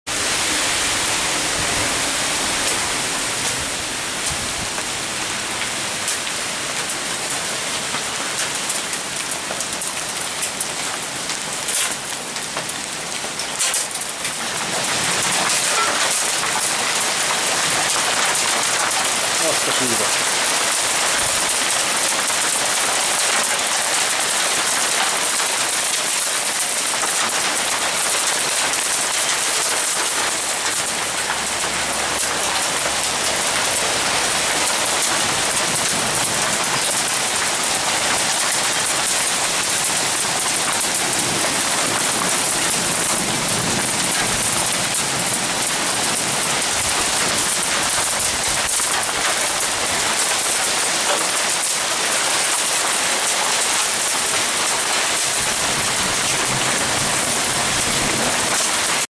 Jégeső Baranya Pécs 2013 06 10
Délután 15 óra 15 perckor, véletlenül kimentem a teraszra. Vittem hamar a kamerát is...